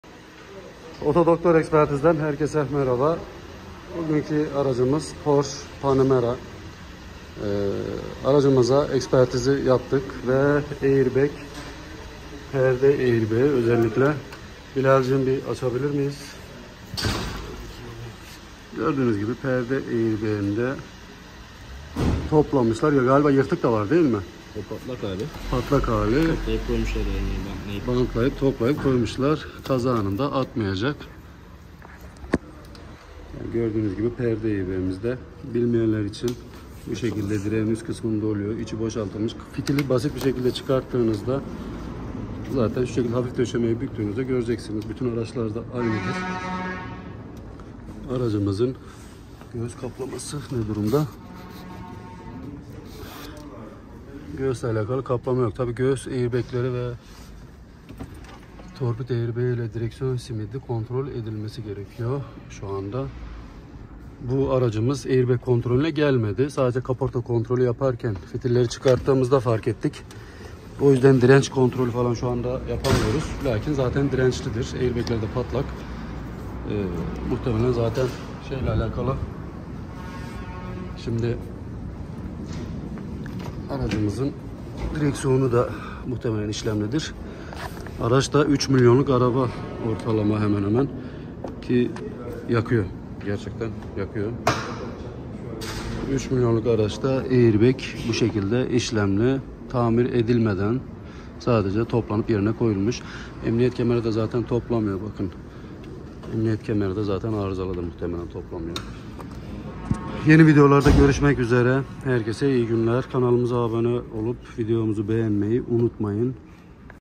3 milyonluk Porsche panamera aırbag açık ve ölüm saçıyor
Milyonluk araba malesef aırbag açık şekilde kullanılıyor bu araçlar ciddi sürat yapabiliyor ve aırbagler çok önemli kazalar ı çok ciddi olduğundan dolayı hayati öneme sahip